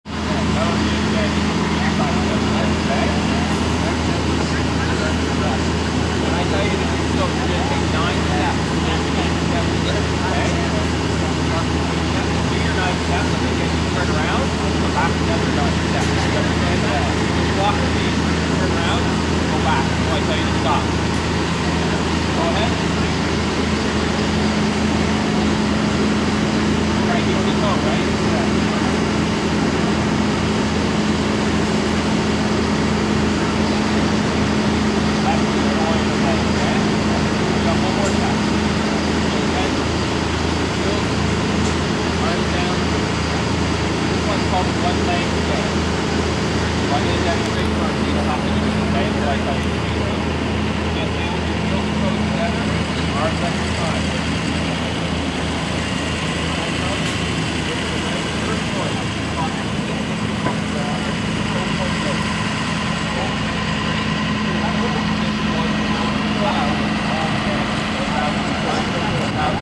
Sobriety Test - Mock Crash, Gravenhurst Highschool
DescriptionOn Wednesday May 20th, a mock crash scenario was held to simulate the dangers of distracted driving and driving under the influence of marijuana to students at Gravenhurst Highschool.

This joint exercise involved many different organizations including: The Gravenhurst Fire Deparment, Chippewas of Rama First Nation Paramedic Services, Local Mental Health Organizations, Gravenhurst High School, the Ontario Provincial Police and the Coroner.

 After the scene was setup, students from Gravenhurst Highschool were brought out to witness the events as they unfolded. Some of the process they witnessed included sobriety tests, deceased victims, injured victims and an arrest.